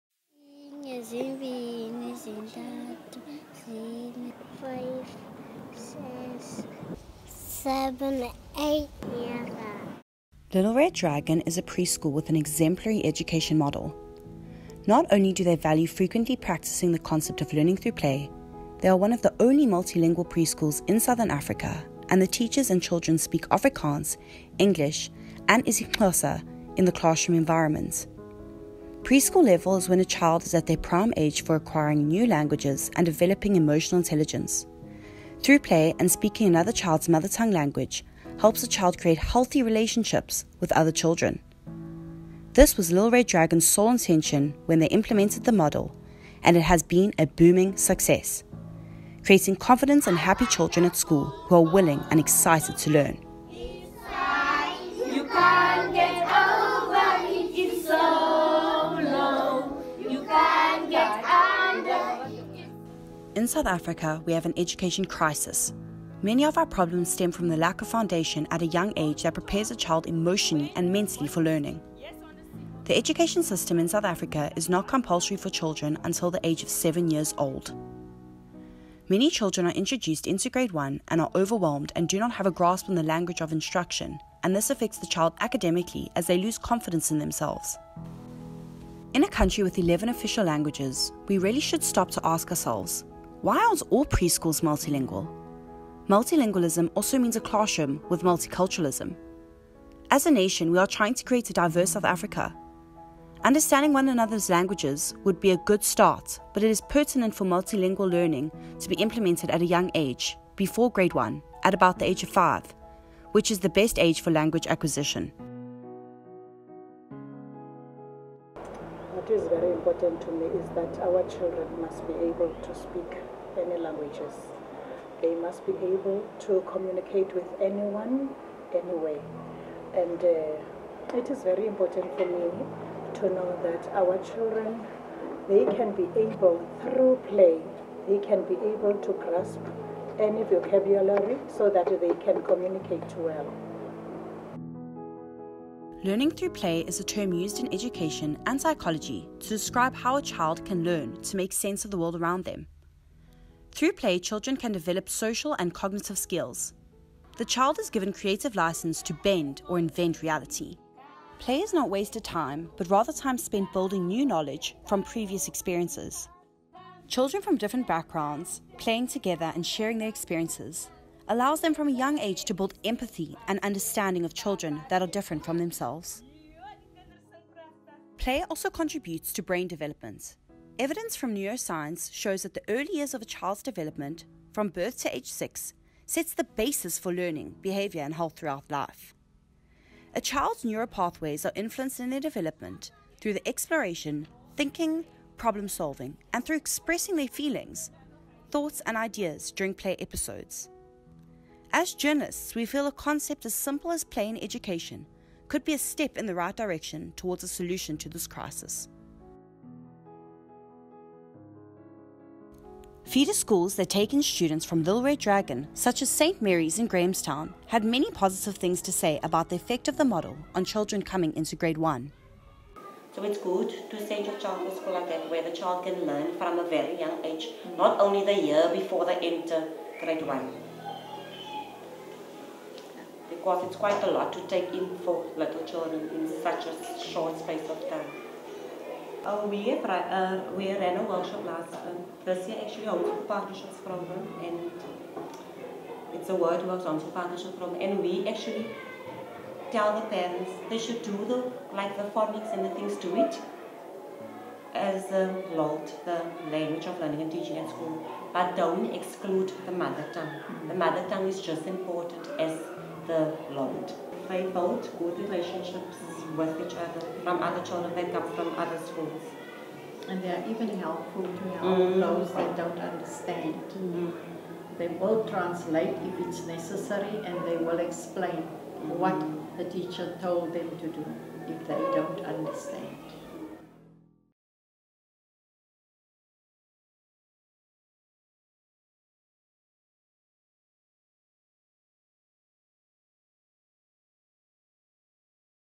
Learning: Little Red Dragon pre-school
We look at an innovative multi-lingual pre-school in Makhanda.

Production by Esihle Faltein & Claire O'Reilly